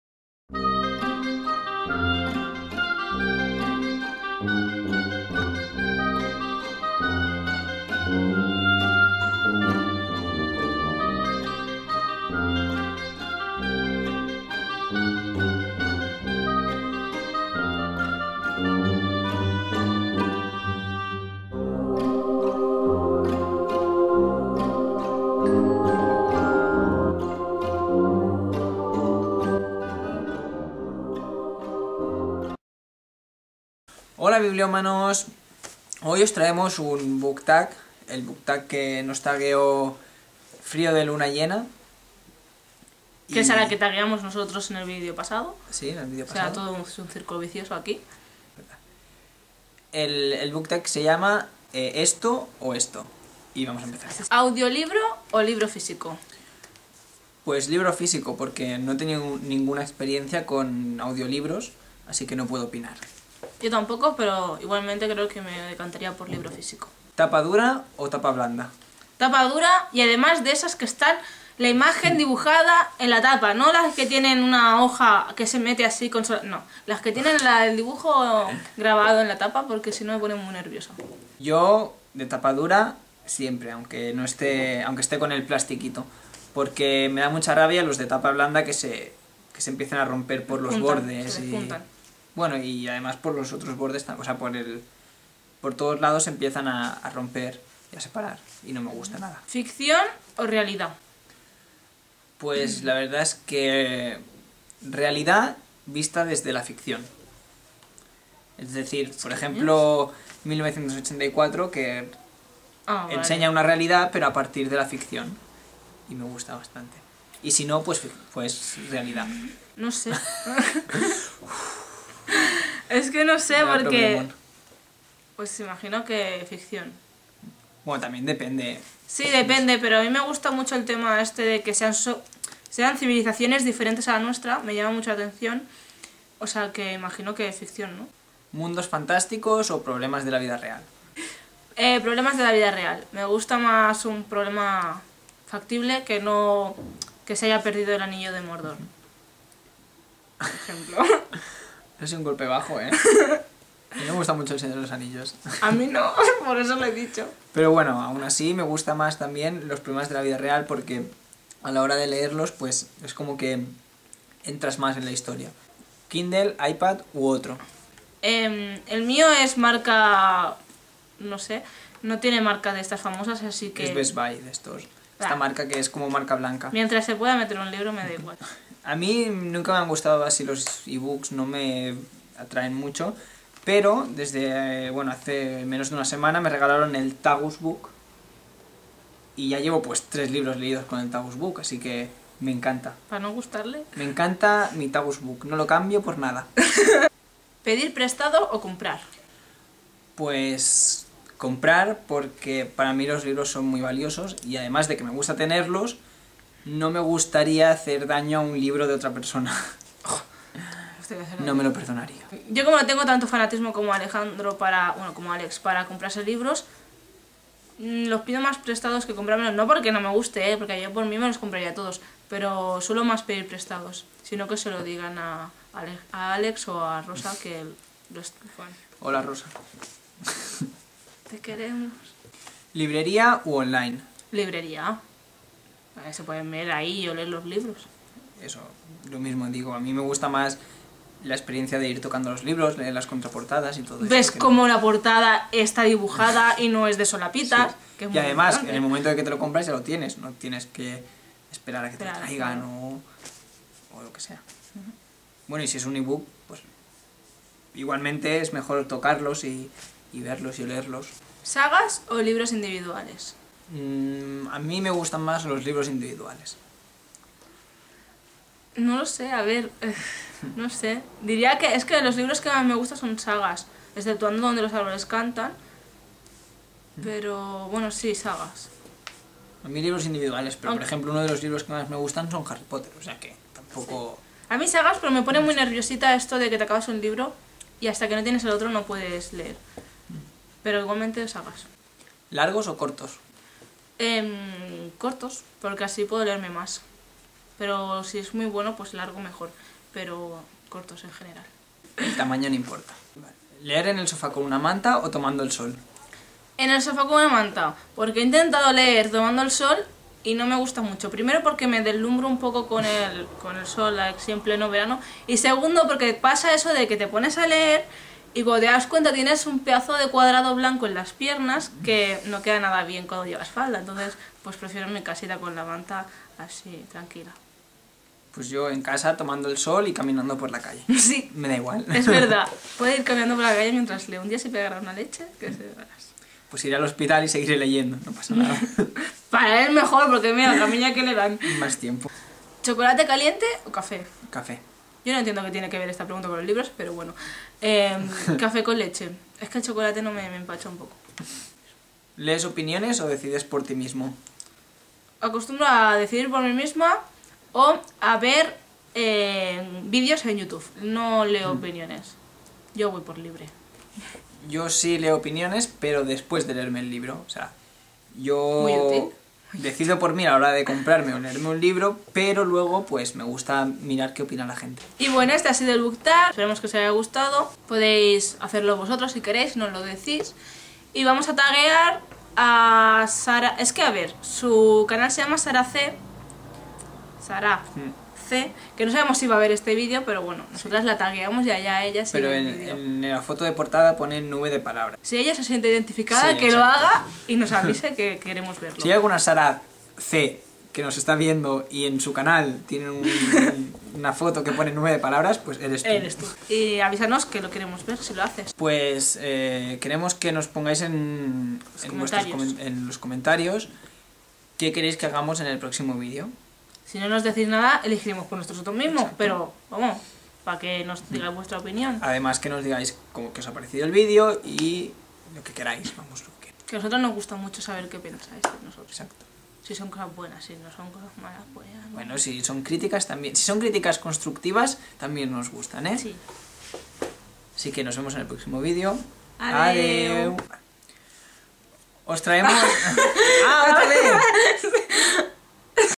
Book tag #4 Esto o Esto
Nuevo Book tag, más escueto que el anterior, que nos quedó algo largo jejej

Esperamos que os agrade y que disfrutéis.